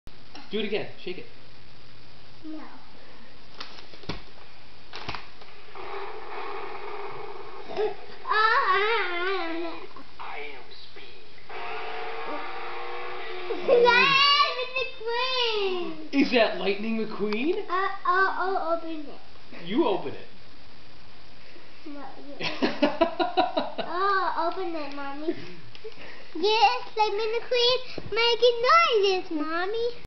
Christmas 2007
Collin opening Lightning McQueen Racecar